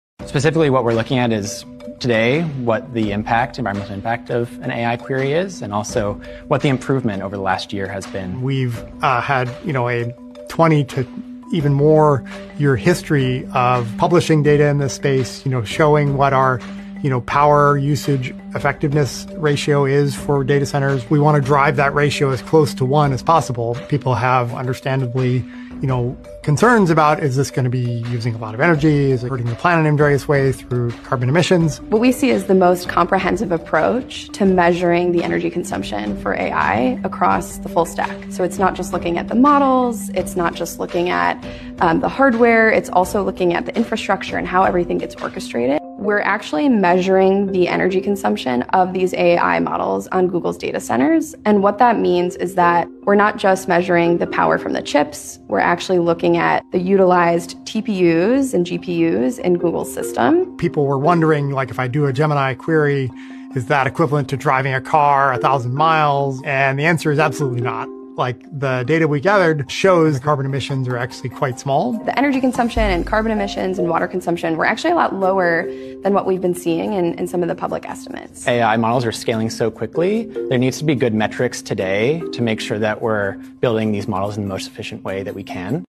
Google has disclosed Gemini’s energy
Google has disclosed Gemini’s energy and water usage, showing lower per-query costs than previously estimated.

While the overall environmental impact is still uncertain, this move reflects a growing trend of transparency and accountability in AI operations.

A step forward in understanding the true cost of advanced AI.